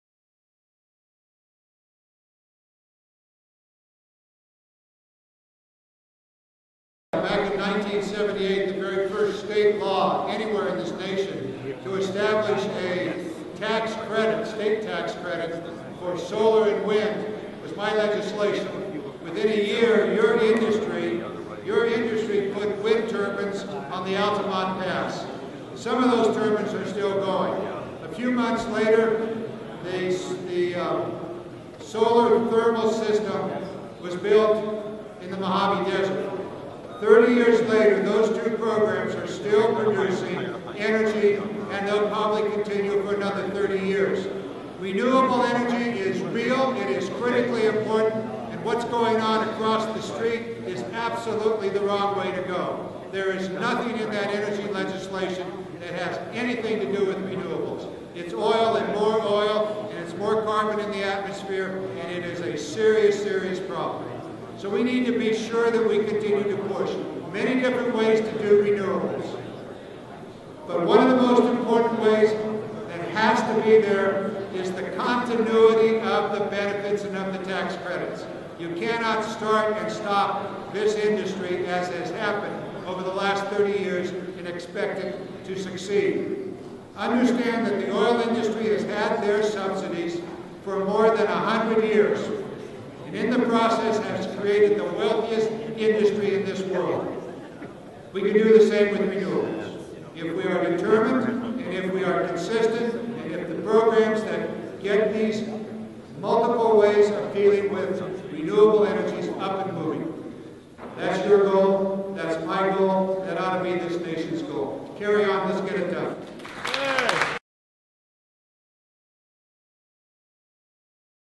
Rep. John Garamendi at the 2012 Congressional Renewable Energy and Energy Efficiency EXPO
Representative John Garamendi (D-CA) spoke at the 15th Annual Congressional Renewable Energy & Energy Efficiency EXPO + Policy Forum on June 21, 2012.